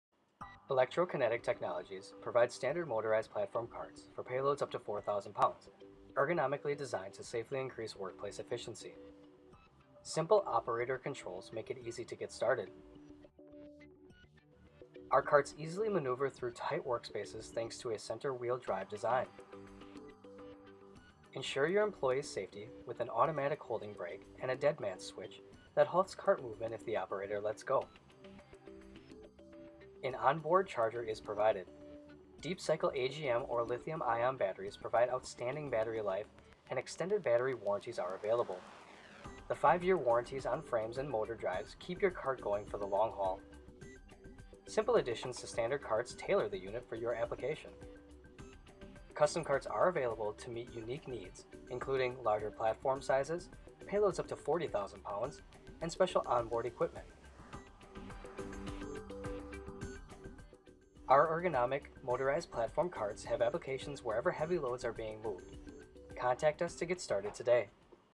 Motorized Platform Carts-Electro Kinetic Technologies
Electro Kinetic Technologies provides standard motorized platform carts for payloads up to 4,000 pounds, ergonomically designed to safely increase workplace efficiency.

Simple operator controls make it easy to get started.  Our carts easily maneuver through tight work spaces thanks to a center wheel drive design.

Ensure your employee’s safety with an automatic holding brake and a dead man’s switch that halts cart movement if the operator lets go.

An on-board charger is provided.  Deep cycle AGM or Lithium Ion batteries provide outstanding battery life, and extended battery warranties are available.  The five-year warranties on frames and motor drives keep your cart going for the long haul.

Simple additions to standard carts tailor the unit for your application.

Custom carts are available to meet unique needs, including larger platform sizes, payloads up to 40,000 pounds, and special on-board equipment.

Our ergonomic motorized platform carts have applications wherever heavy loads are being moved.